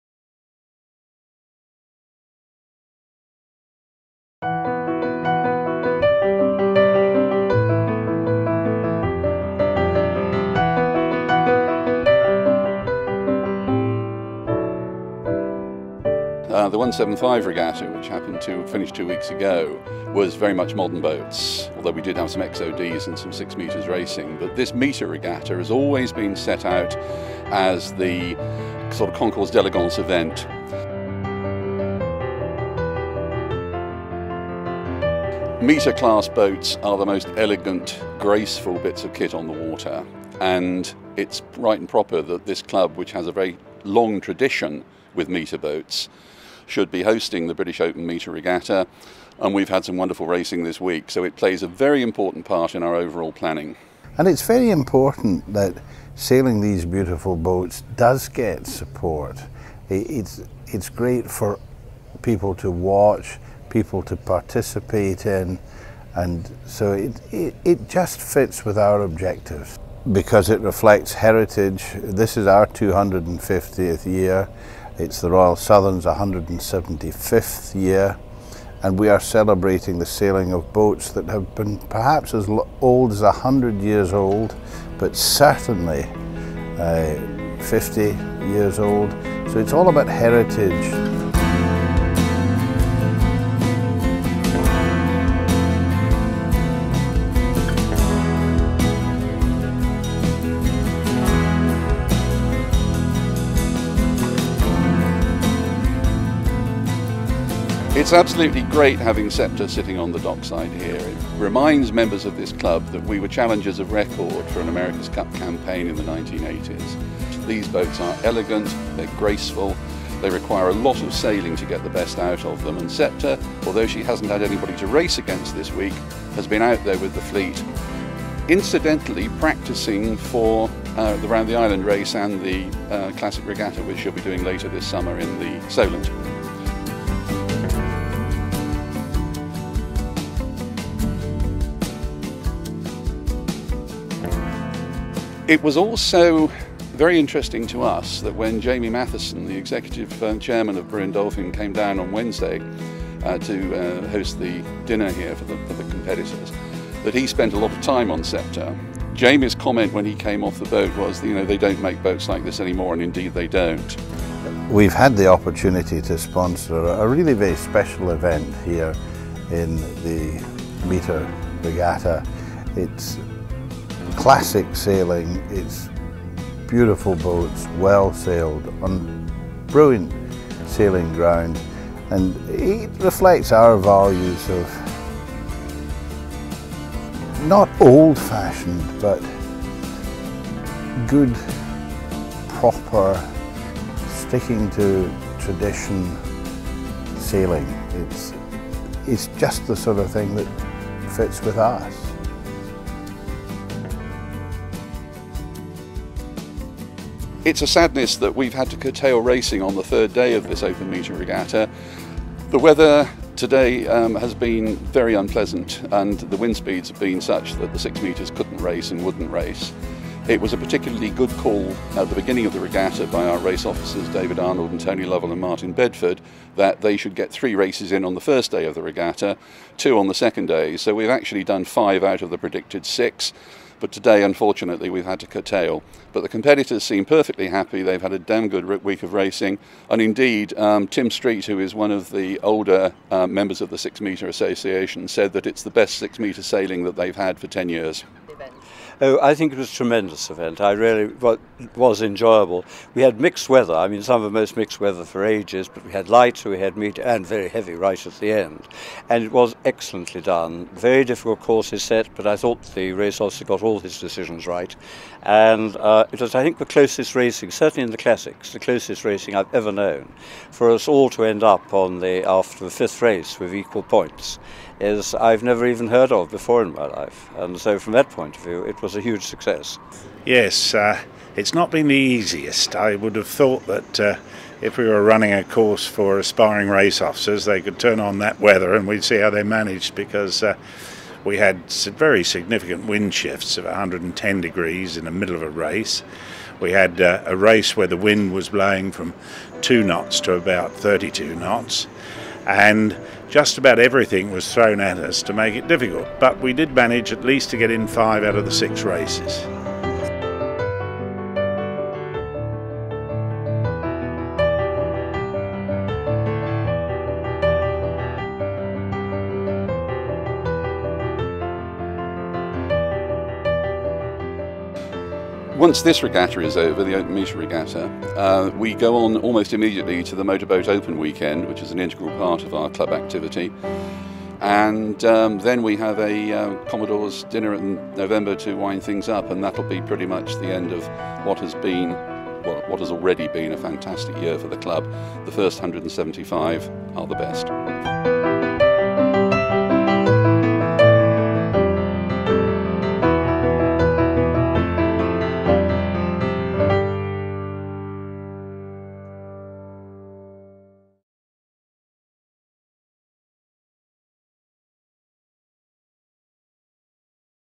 Royal Southern yacht Club 175 PT3
The Royal Southern Yacht Club is celebrating 175 years in 2012. Over the 12 months they have been planning events, parties and regattas.  In the third of 3 videos we take a look at some of the great action on and off the water.